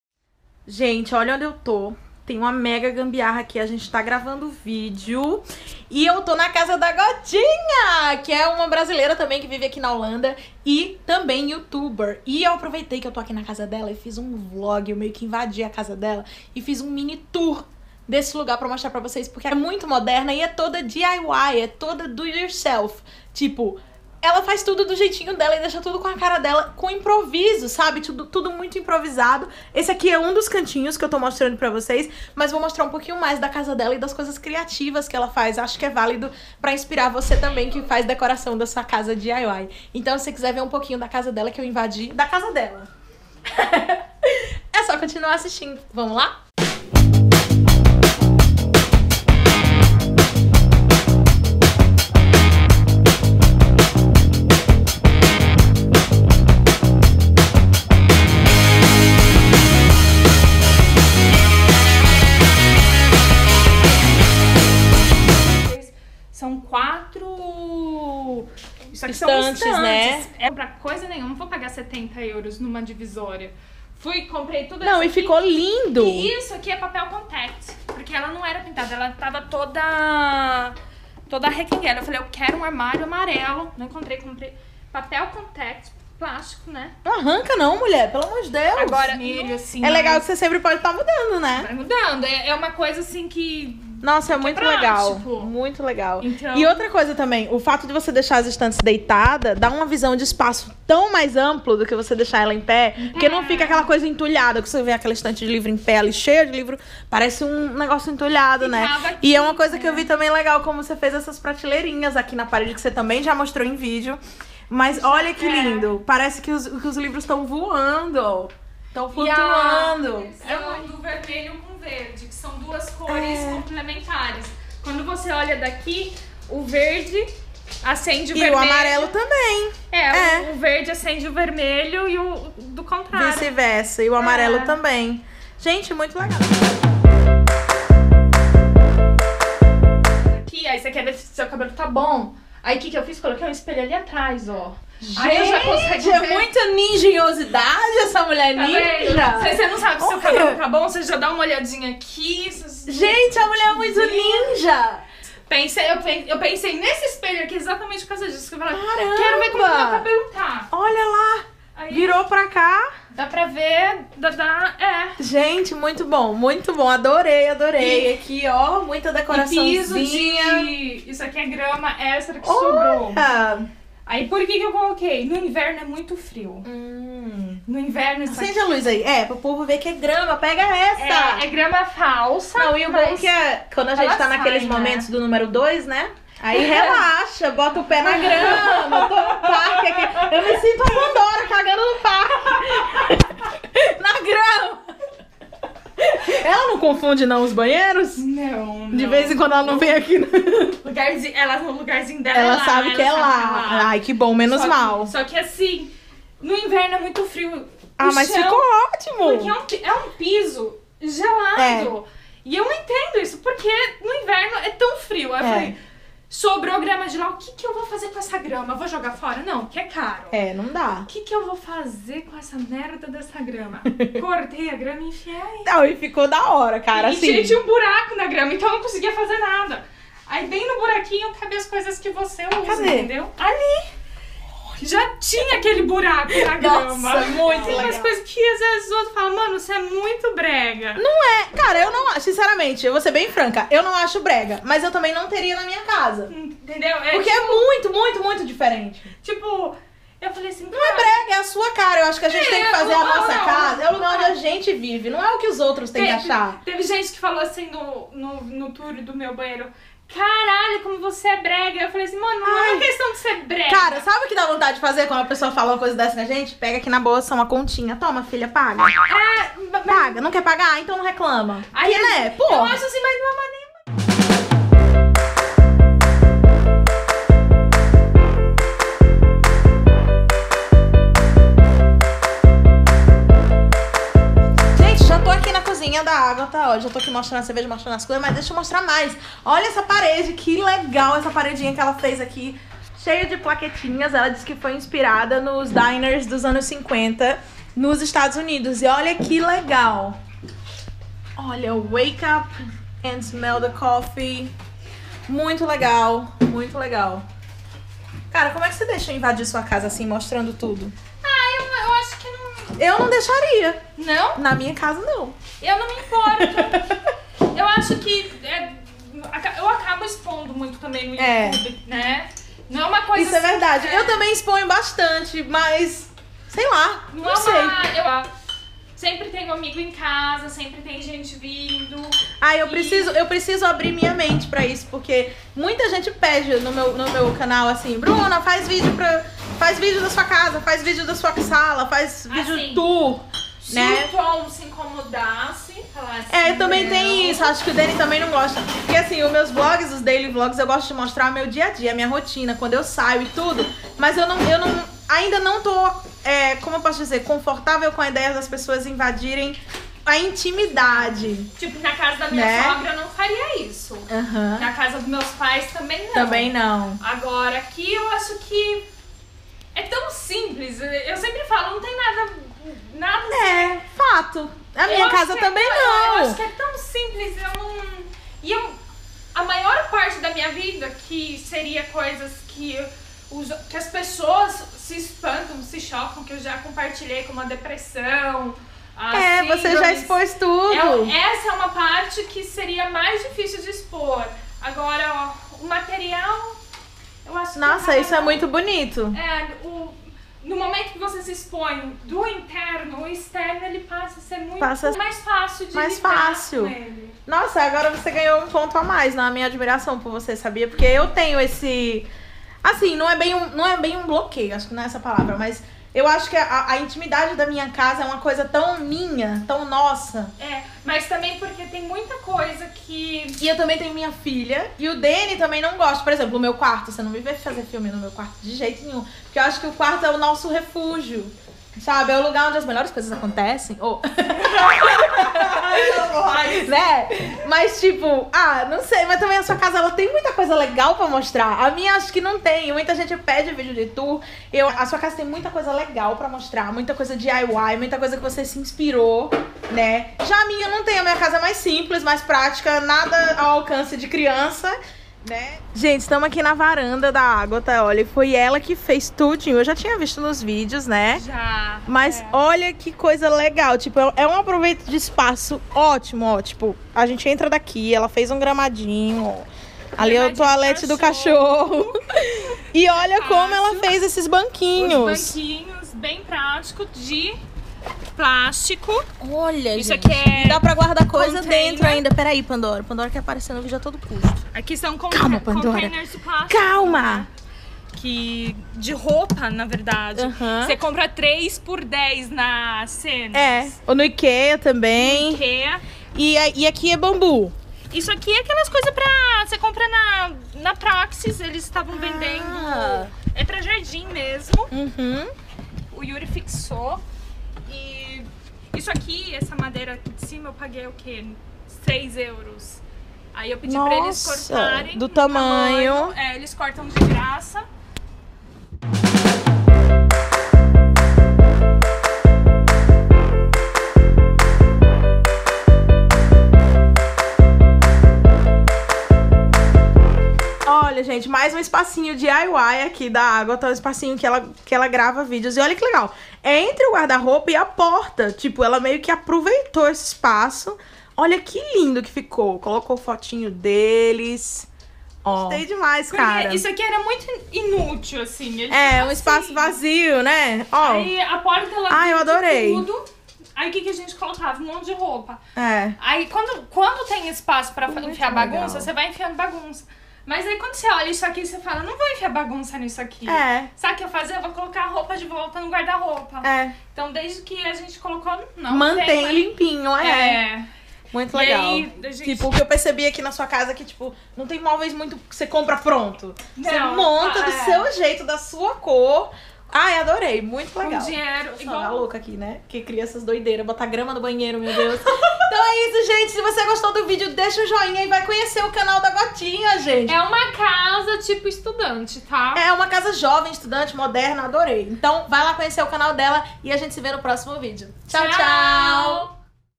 Cagando na grama? Tour pela casa mais DIY que eu já vi. Feat. Gothinha
Hoje eu invadi a casa da Gothinha para mostrar suas ideias geniais de aproveitamento de espaços, e decoração DIY.

No vídeo eu também conto por que ainda não fiz tour pela minha própria casa. Sejam bem vindos!

Facebook.com/brunnacomdoisns